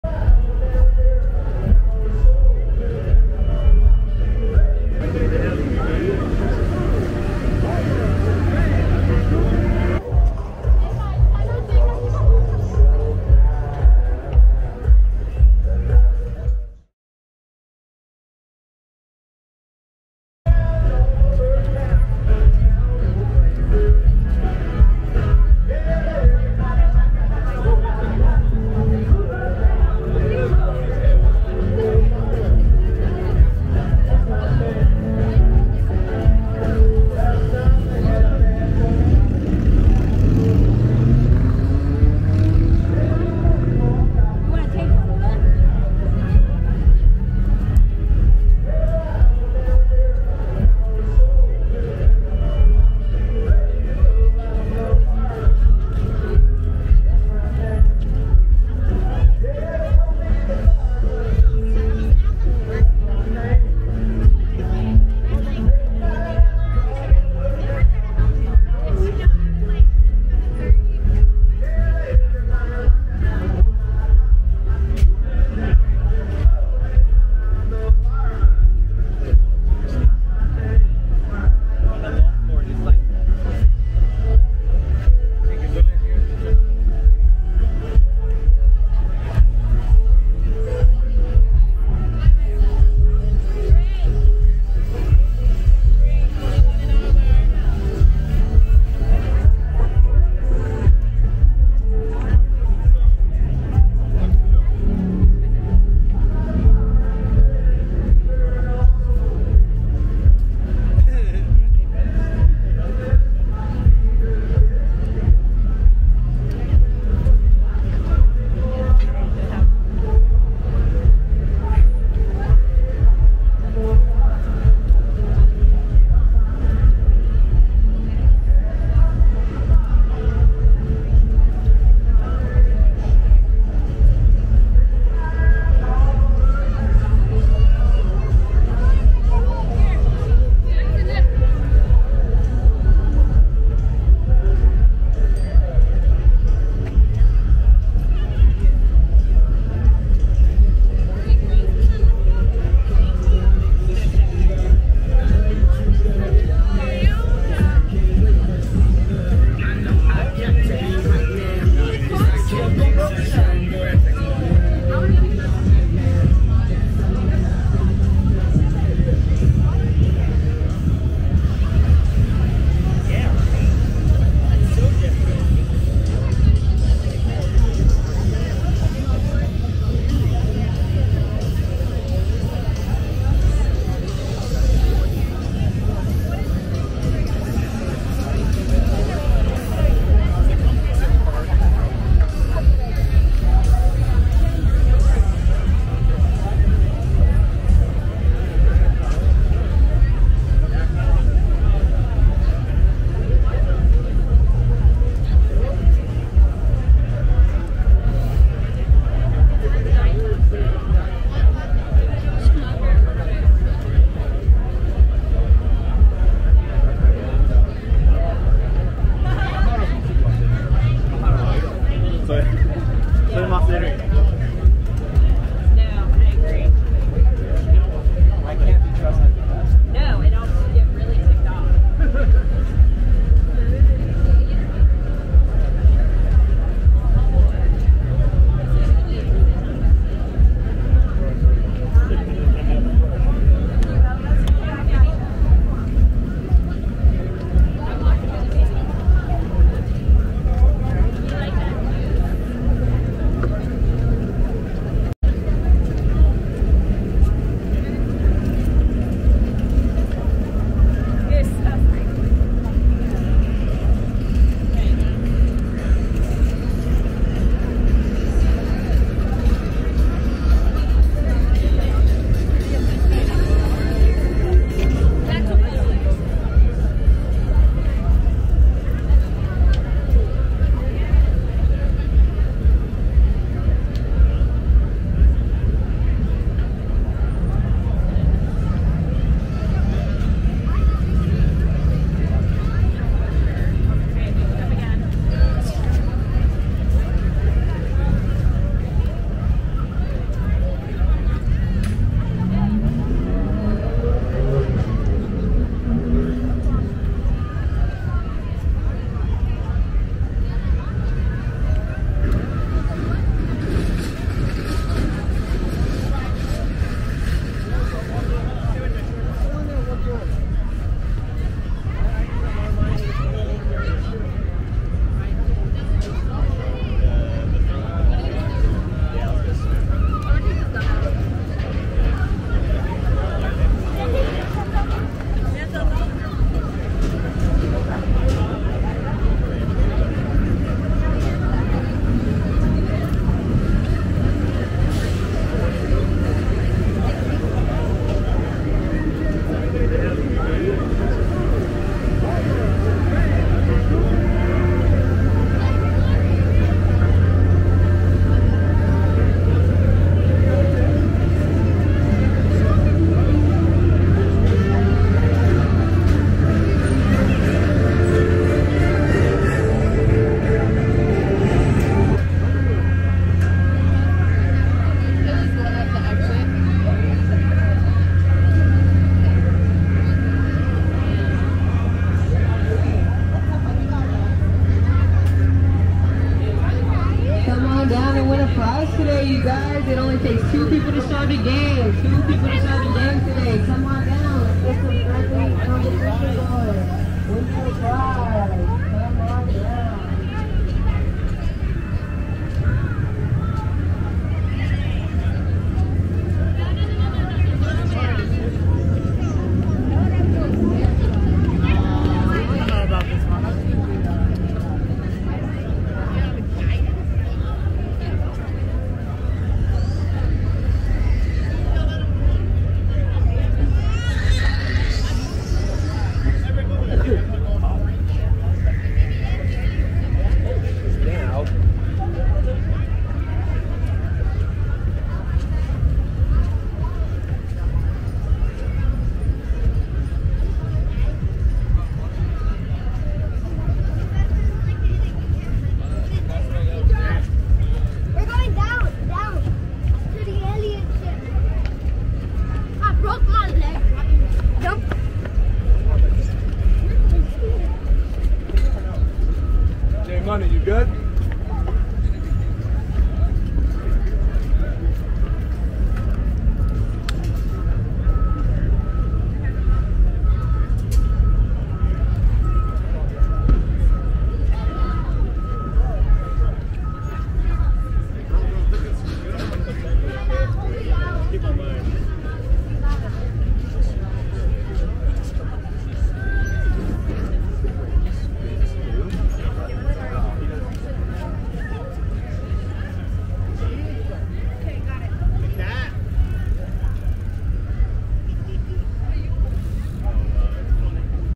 4th of July Fair at Huntington Beach 2022
Walking Tour of the July 4th Fair at Huntington Beach 2022.
Great way to celebrate Independence Day holiday!

"Launched in 1904, the first Huntington Beach 4th of July celebration commemorated the arrival of the first electric passenger train linking the area with Long Beach and Los Angeles, attracting 50,000 people to the celebration.

118 years later, the annual tradition annually attracts more than 500,000 attendees over 4 days of programming and is the largest 4th of July celebration west of the Mississippi"